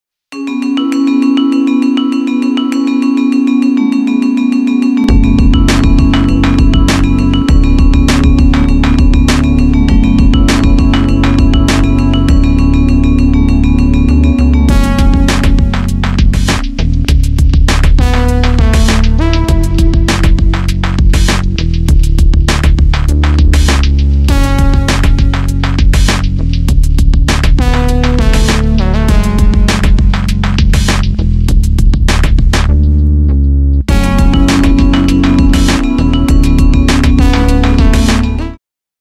Perth Australia Epilepsy Visual - by Arfad
An experimental visual representation of Perth Australia